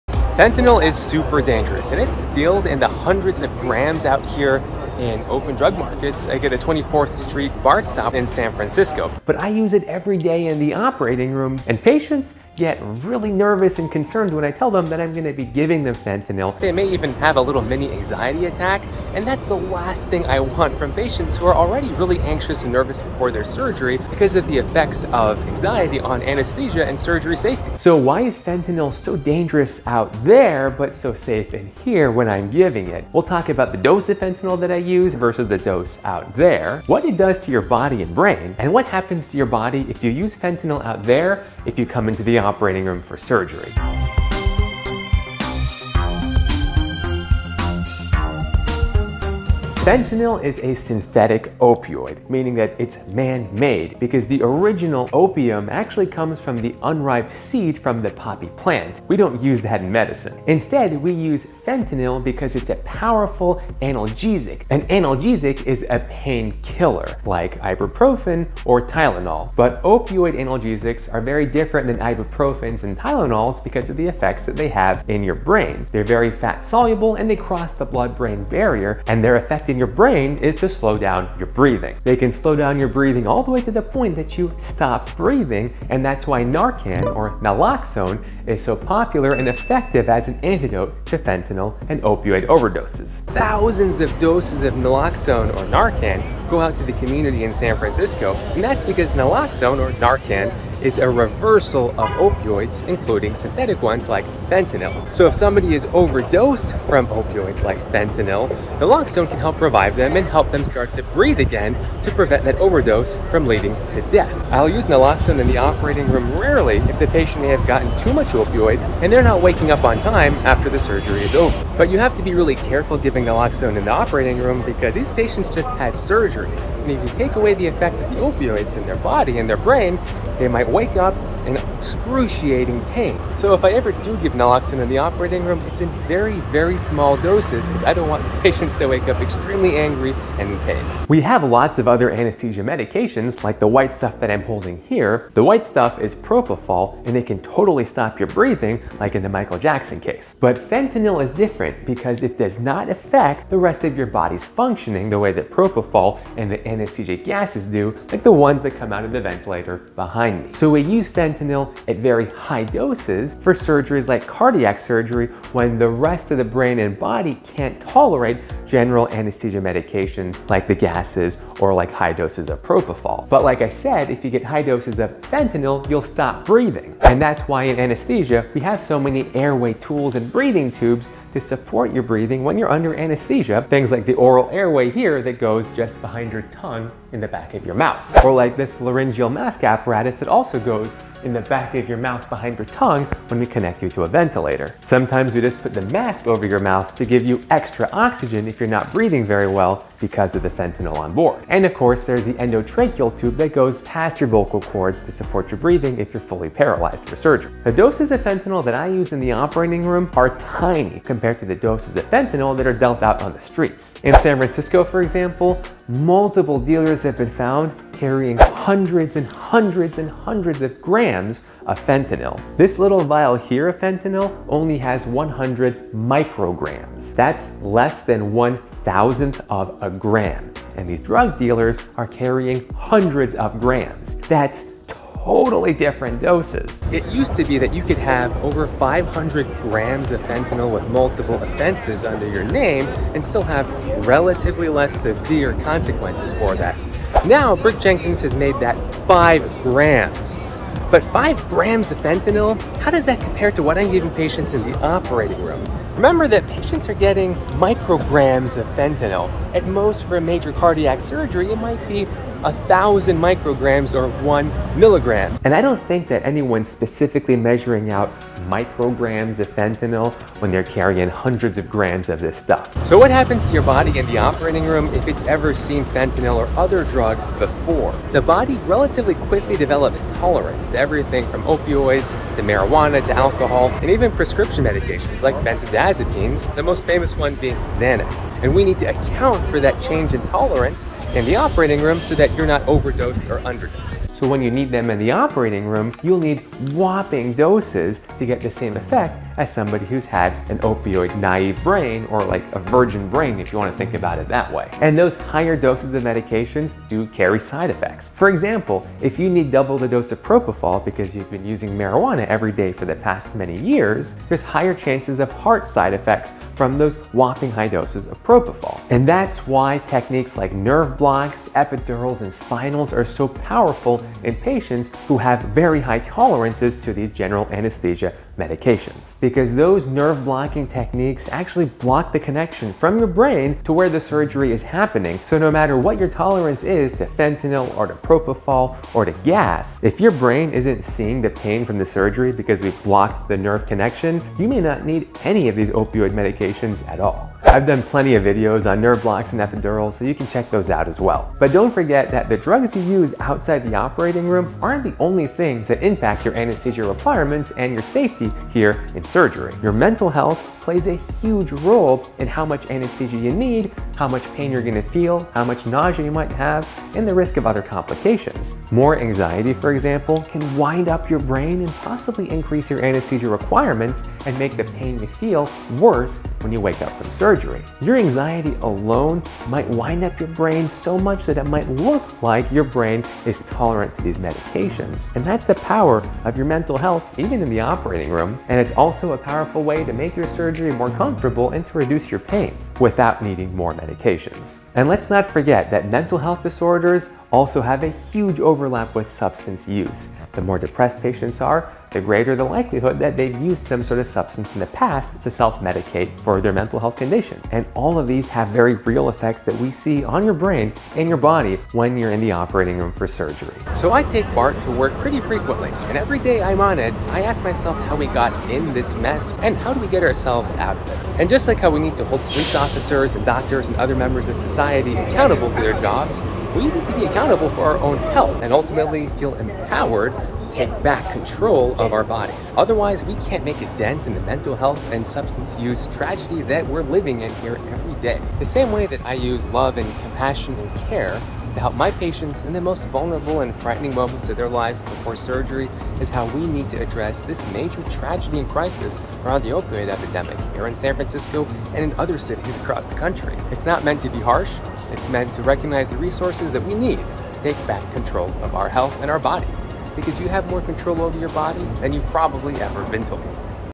Fentanyl Overdose: Street vs Surgery (and San Francisco Opioid Crisis)
The opioid epidemic is related to pain medication used in surgery. Most patients don't know they're receiving fentanyl under anesthesia. Here's the difference between surgery and the streets.

Fentanyl is one of the most widely used and abused medications, causing overdose both on the streets and in the operating room. It is a synthetic opioid that has become very cheap in recent years and has spilled on the streets of San Francisco and the rest of the United States, well outside of the operating room where I use it safely every day in surgery for anesthesia.

I practice anesthesia and integrative medicine in the San Francisco Bay Area, where the opioid epidemic has caused countless deaths from overdose, over 150 per month. Fentanyl has caused a plurality of these overdoses and deaths, particularly in San Francisco. It breaks my heart to see how tragic an effect such a powerful medication has in the wrong hands without the proper training.

The problem has become so severe that my patients often get anxious and scared before surgery when they hear that I will be giving them fentanyl for their anesthesia. This extra anxiety and fear can cause complications in the operating room and in recovery after surgery.

I want to reduce the anxiety and fears that my patients have before surgery, because mental health is so important to our wellbeing and physical health inside and outside the operating room. That's why we will discuss the dose of fentanyl used in surgery versus the dose used on the street, what it actually does to your body, and ultimately why it is safe and necessary to use in the operating room, but so dangerous on the streets.

We will also discuss the steps San Francisco has taken to reduce the opioid overdose crisis from fentanyl under District Attorney Brooke Jenkins. Overcoming the opioid epidemic, particularly in San Francisco, is a complicated problem. The solution will heavily rely on all of us being accountable to each other as much as we are accountable to ourselves. We have more power to control our health than we have ever been told, and with that accountability and responsibility for our health is great hope for saving lives and living safer.